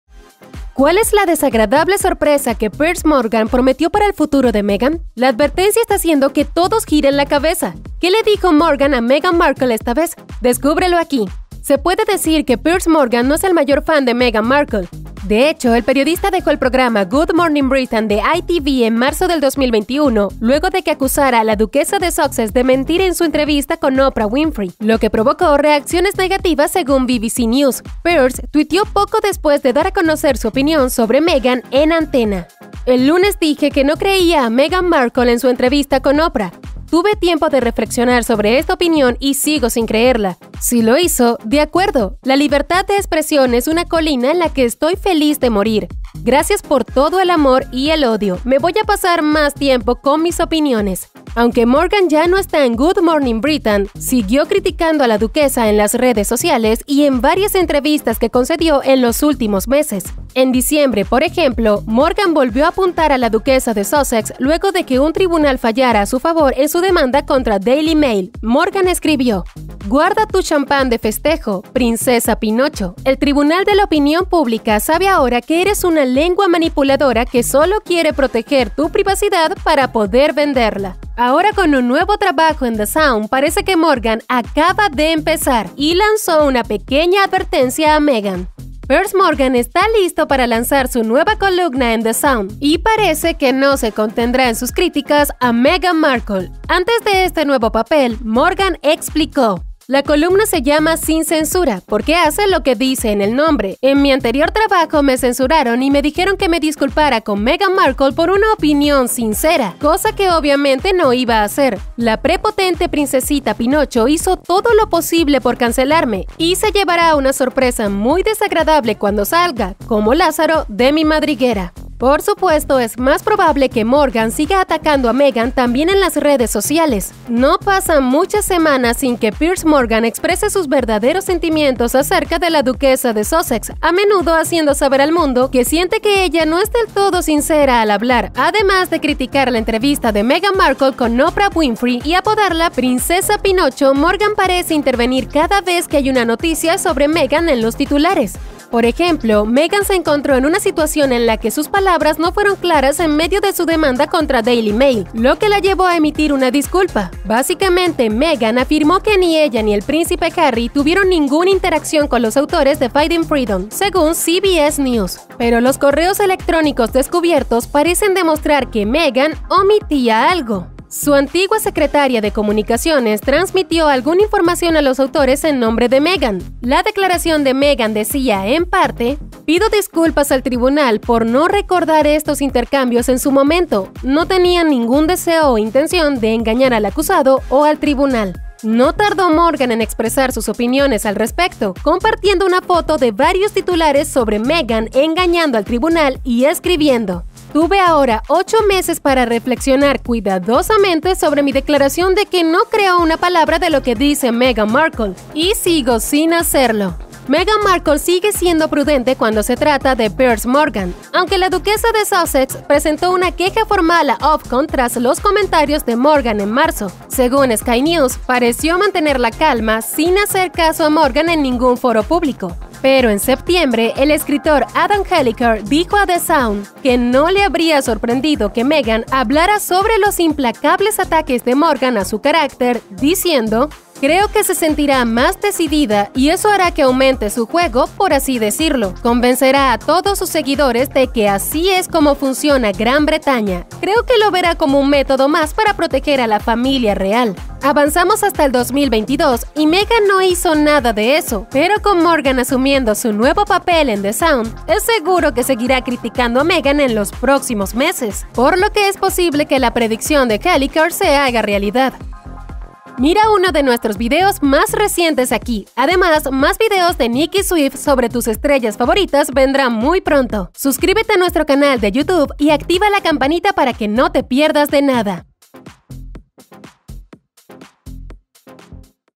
Piers Morgan Lanza Desconcertante Advertencia A Meghan Markle
¿Cuál es la "desagradable sorpresa" que Piers Morgan prometió para el futuro de Meghan? La advertencia está haciendo que todos giren la cabeza. ¿Qué le dijo Morgan a Meghan Markle esta vez? Descúbrelo aquí.

Se puede decir que Piers Morgan no es el mayor fan de Meghan Markle. De hecho, el periodista dejó el programa Good Morning Britain de ITV en marzo del 2021 luego de que acusara a la duquesa de Sussex de mentir en su entrevista con Oprah Winfrey, lo que provocó reacciones negativas, según BBC News. Piers twitteó poco después de dar a conocer su opinión sobre Meghan en antena:

"El lunes dije que no creía a Meghan Markle en su entrevista con Oprah. Tuve tiempo de reflexionar sobre esta opinión y sigo sin creerla. Si lo hizo, de acuerdo. La libertad de expresión es una colina en la que estoy feliz de morir. Gracias por todo el amor, y el odio. Me voy a pasar más tiempo con mis opiniones".

En diciembre, por ejemplo, Morgan volvió a apuntar a la duquesa de Sussex, luego de que un tribunal fallara a su favor en su demanda contra Daily Mail. Morgan escribió:

"Guarda tu champán de festejo, Princesa Pinocho: el tribunal de la opinión pública sabe ahora que eres una lengua manipuladora que solo quiere proteger tu privacidad para poder venderla".

Ahora, con un nuevo trabajo en The Sun, parece que Morgan acaba de empezar, y lanzó una pequeña advertencia a Meghan.

Piers Morgan está listo para lanzar su nueva columna en The Sun, y parece que no se contendrá en sus críticas a Meghan Markle. Antes de este nuevo papel, Morgan explicó:

"La columna se llama 'Sin censura' porque hace lo que dice en el nombre. En mi anterior trabajo me censuraron y me dijeron que me disculpara con Meghan Markle por una opinión sincera, cosa que obviamente no iba a hacer. La prepotente princesita Pinocho hizo todo lo posible por cancelarme, y se llevará una sorpresa muy desagradable cuando salga, como Lázaro, de mi madriguera".

No pasan muchas semanas sin que Piers Morgan exprese sus verdaderos sentimientos acerca de la duquesa de Sussex, a menudo haciendo saber al mundo que siente que ella no es del todo sincera al hablar . Además de criticar la entrevista de Meghan Markle con Oprah Winfrey y apodarla "Princesa Pinocho", Morgan parece intervenir cada vez que hay una noticia sobre Meghan en los titulares.

Por ejemplo, Meghan se encontró en una situación en la que sus palabras no fueron claras en medio de su demanda contra Daily Mail, lo que la llevó a emitir una disculpa. Básicamente, Meghan afirmó que ni ella ni el Príncipe Harry tuvieron ninguna interacción con los autores de Finding Freedom, según CBS News. Pero los correos electrónicos descubiertos parecen demostrar que Meghan omitía algo. Su antigua secretaria de comunicaciones transmitió alguna información a los autores en nombre de Meghan. La declaración de Meghan decía, en parte:

"Pido disculpas al tribunal por no recordar estos intercambios en su momento. No tenía ningún deseo o intención de engañar al acusado o al tribunal".

No tardó Morgan en expresar sus opiniones al respecto, compartiendo una foto de varios titulares sobre Meghan engañando al tribunal, y escribiendo:

"Tuve ahora 8 meses para reflexionar cuidadosamente sobre mi declaración de que no creo una palabra de lo que dice Meghan Markle... y sigo sin hacerlo".

Meghan Markle sigue siendo prudente cuando se trata de Piers Morgan. Aunque la duquesa de Sussex presentó una queja formal a Ofcom tras los comentarios de Morgan en marzo, según Sky News, pareció mantener la calma, sin hacer caso a Morgan en ningún foro público. Pero en septiembre, el escritor Adam Helliker dijo a The Sun que no le habría sorprendido que Meghan hablara sobre los implacables ataques de Morgan a su carácter, diciendo:

"Creo que se sentirá más decidida y eso hará que aumente su juego, por así decirlo. Convencerá a todos sus seguidores de que así es como funciona Gran Bretaña. Creo que lo verá como un método más para proteger a la Familia Real".

Avanzamos hasta el 2022 y Meghan no hizo nada de eso. Pero con Morgan asumiendo su nuevo papel en The Sun, es seguro que seguirá criticando a Meghan en los próximos meses, por lo que es posible que la predicción de Helliker se haga realidad.